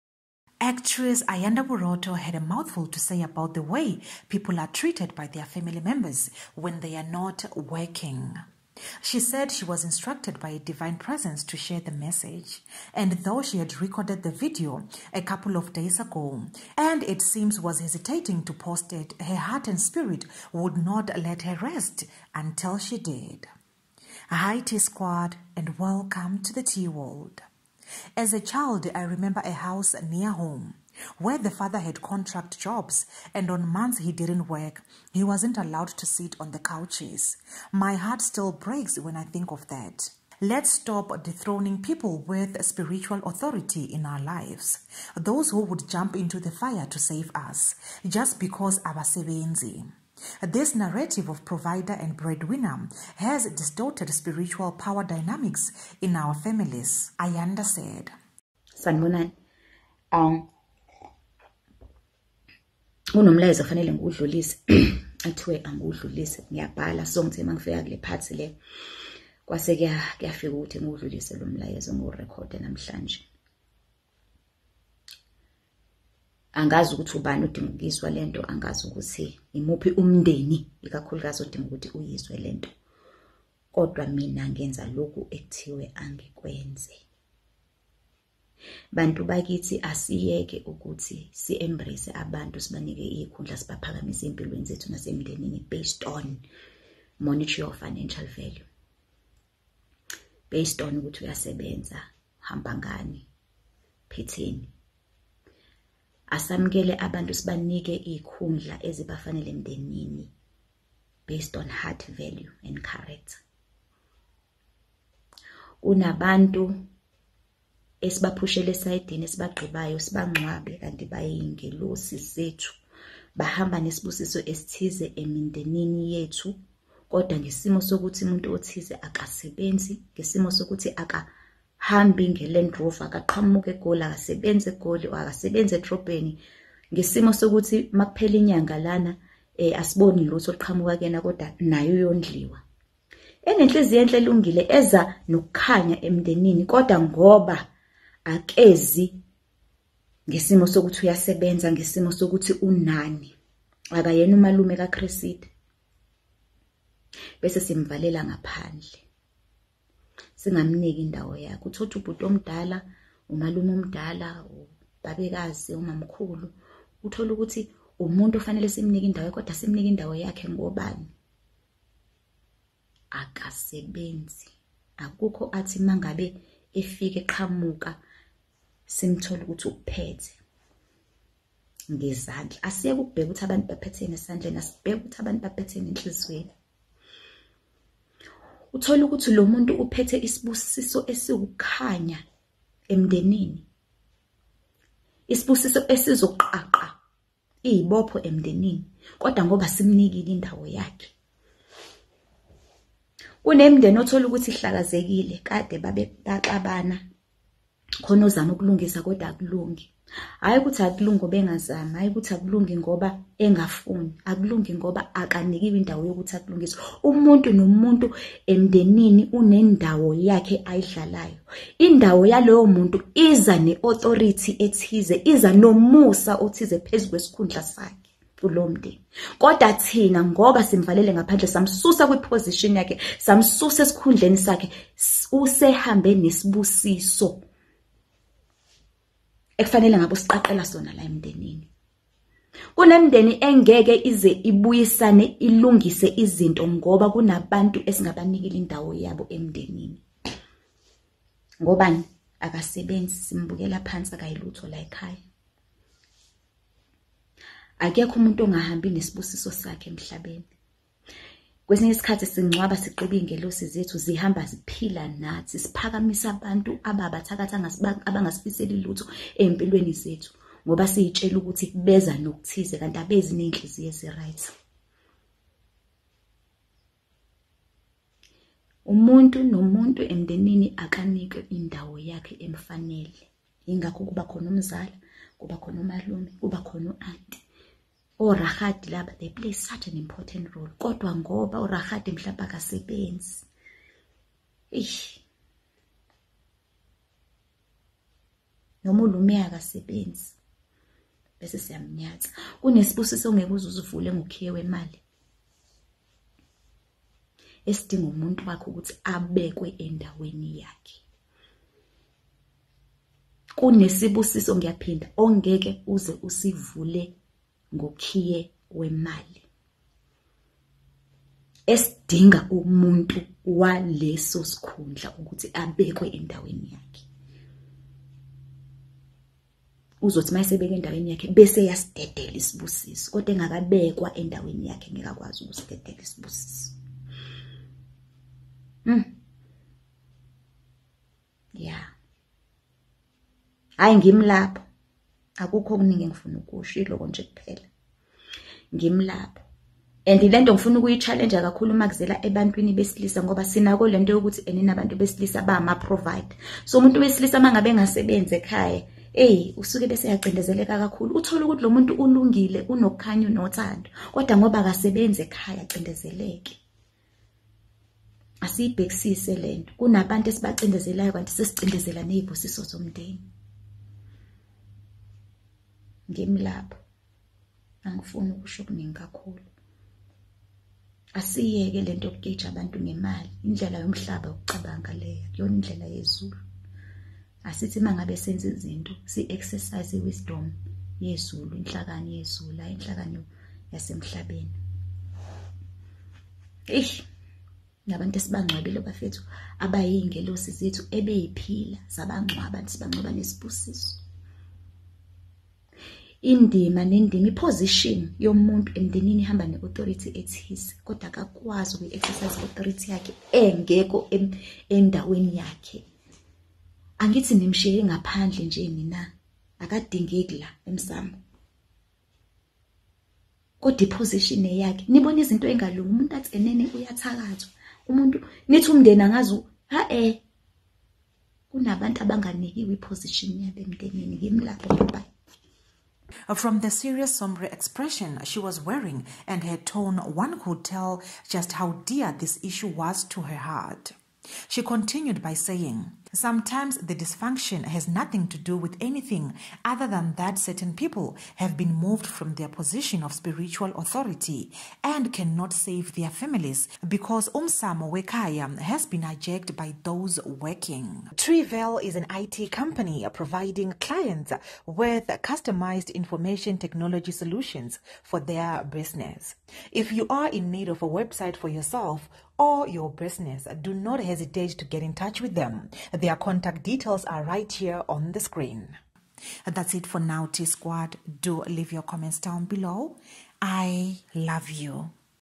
Ayanda Borotho Rebukes Her Fans
Ayanda Borotho
Actress Ayanda Borotho had a mouthful to say about the way people are treated by their family members when they are not working. She said she was instructed by a divine presence to share the message. And though she had recorded the video a couple of days ago and it seems was hesitating to post it her heart and spirit would not let her rest until she did.
“As a child I remember a house near home, where the father had contract jobs and on months he didn’t work, he wasn’t allowed to sit on the couches. My heart still breaks when I think of that. Let’s top dethroning people with spiritual authority in our lives (those who would jump into the fire to save us) just because abasebenzi